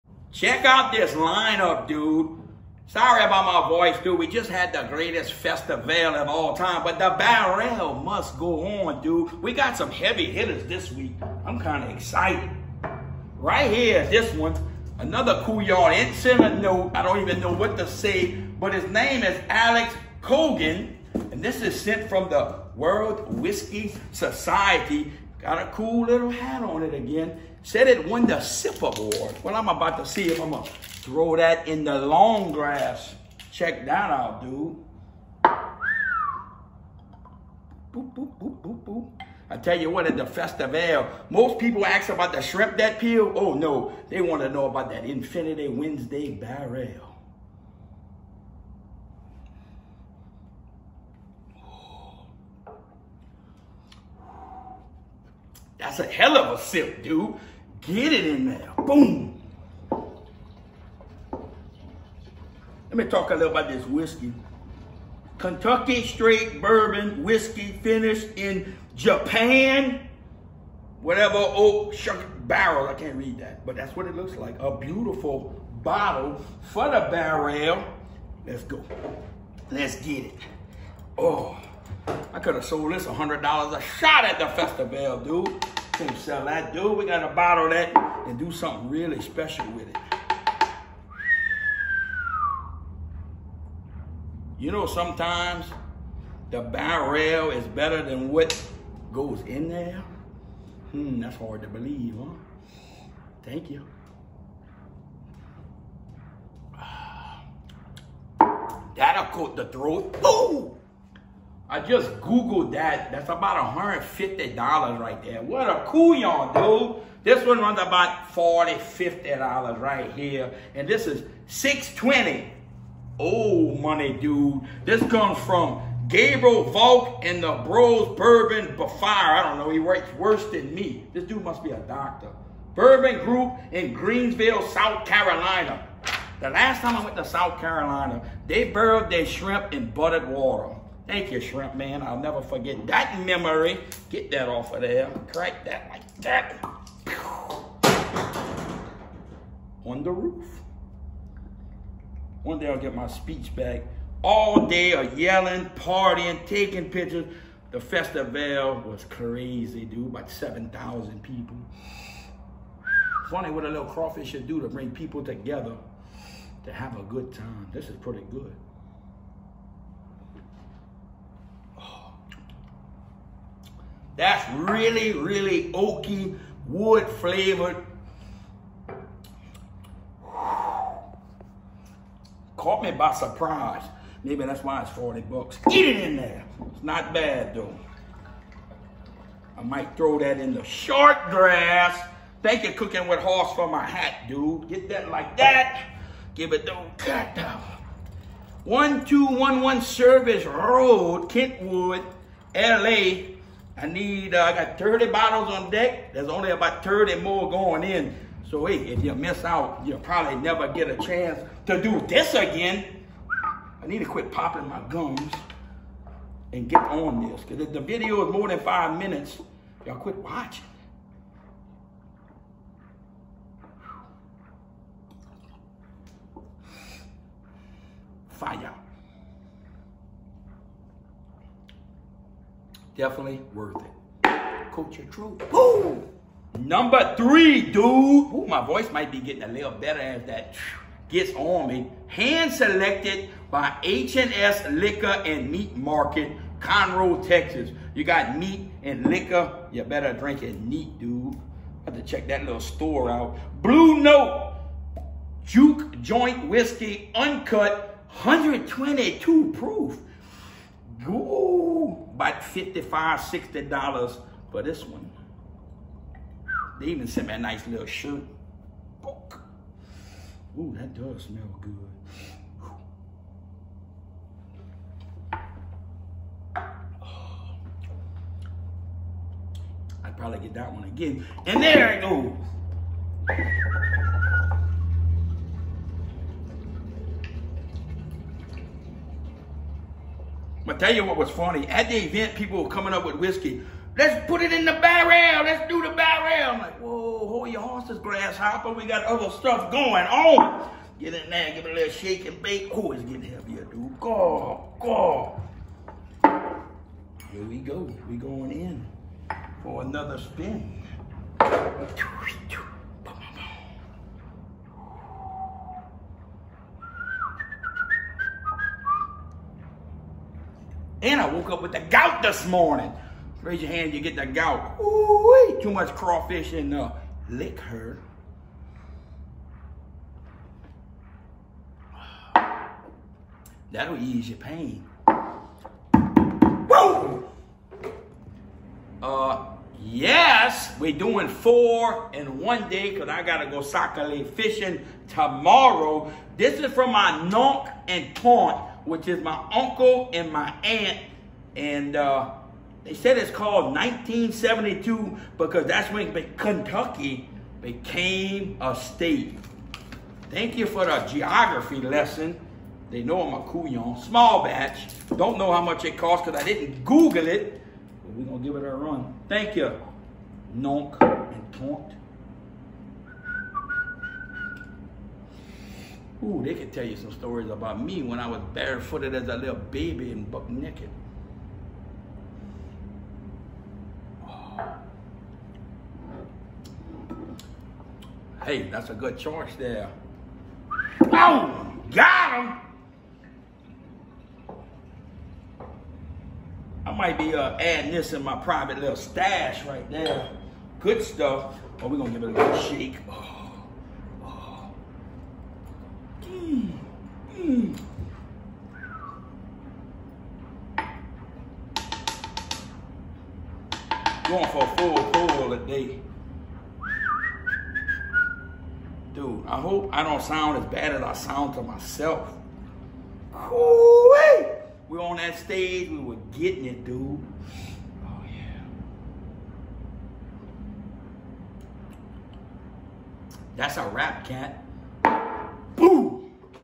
Infinity Barrel Wednesday!!
Had a great time last weekend at the Cajun2step fest!! Time to sit back and recover with the ole infinity barrel dude! Straight money! 1211 service rd Kentwood la cajun